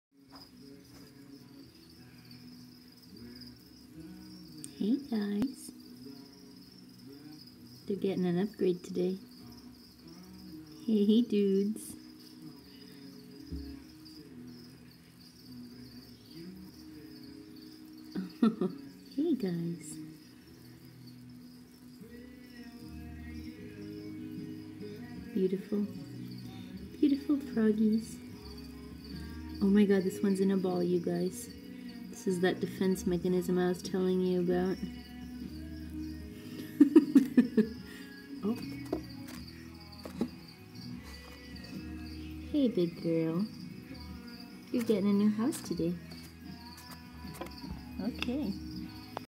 Finding Bronze Auratus holdbacks in a huddled pile before being moved out of the grow out viv
This is a little pile of my Bronze Auratus holdbacks. I was looking for them inside their grow out enclosure in order to move them into a larger enclosure and found them huddled in this little pile. They are so adorable!! 😍